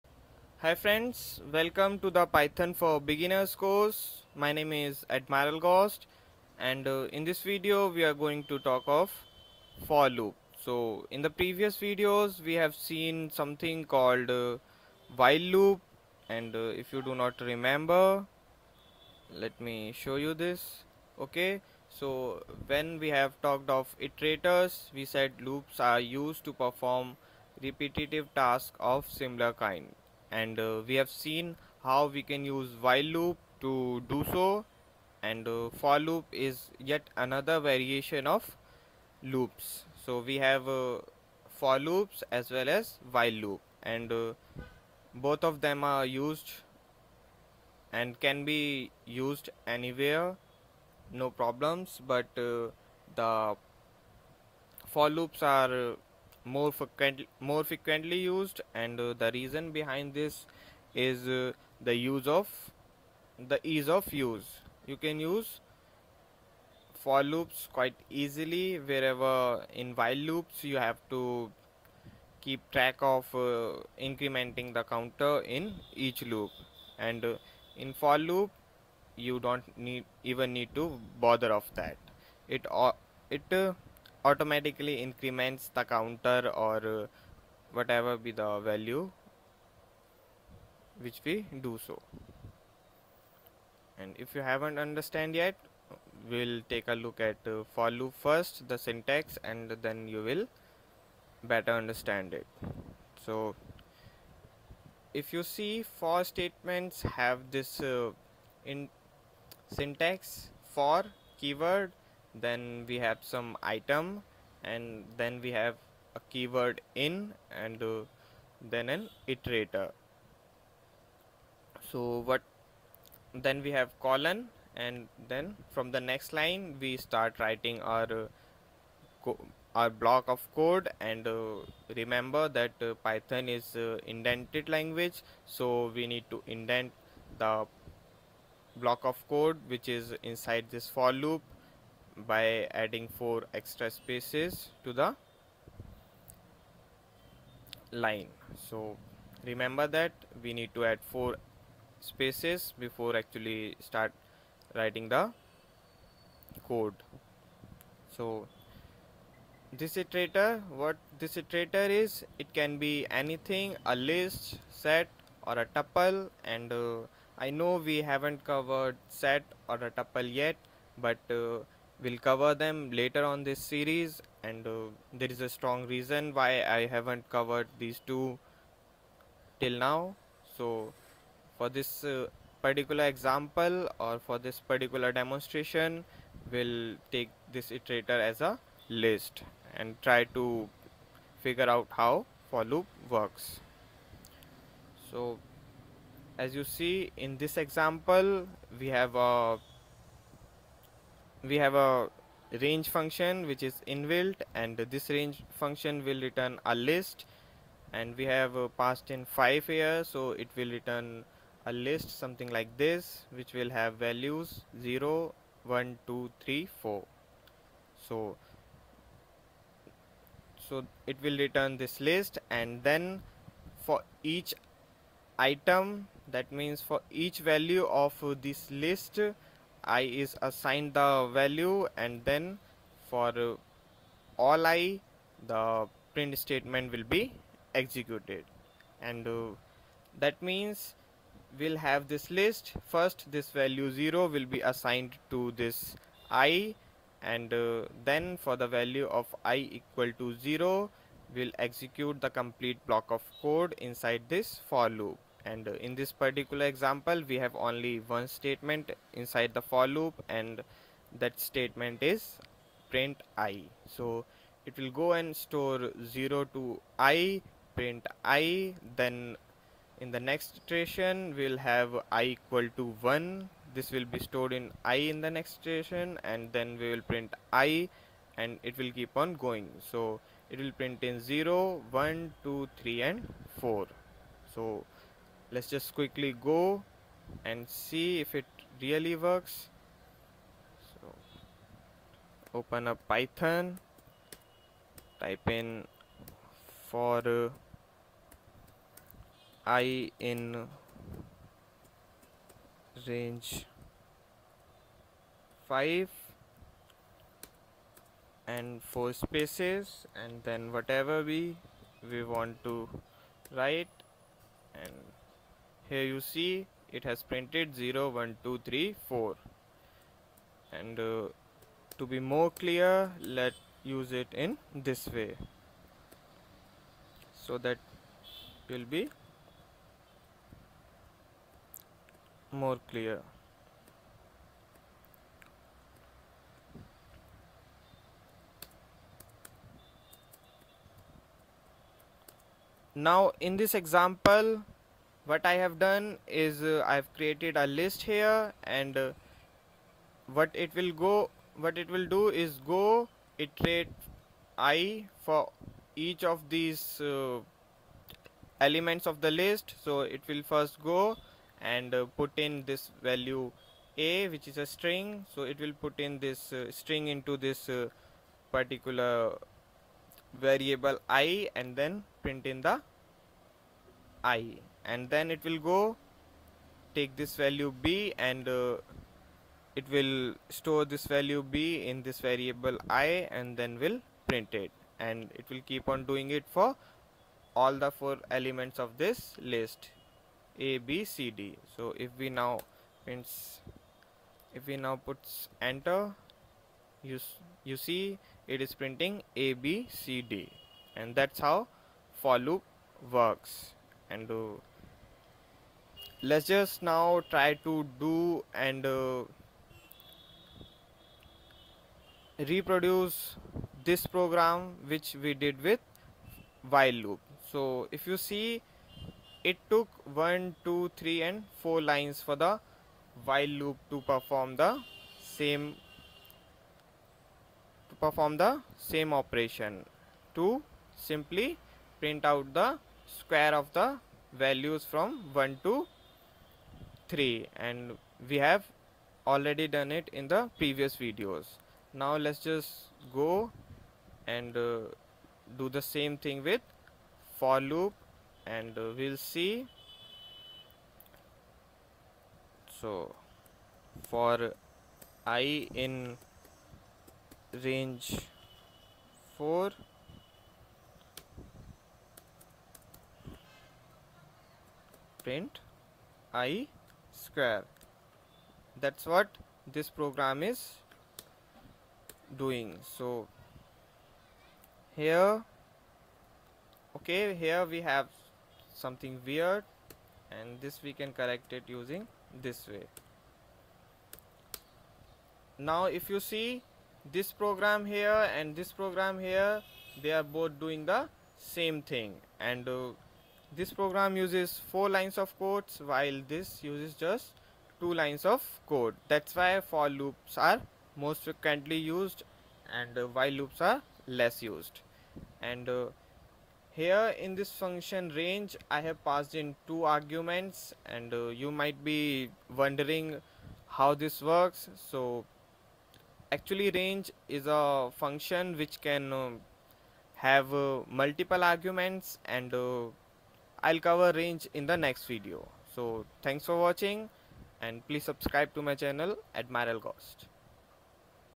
For loop
This video demonstrates the use of foor loops in python programming.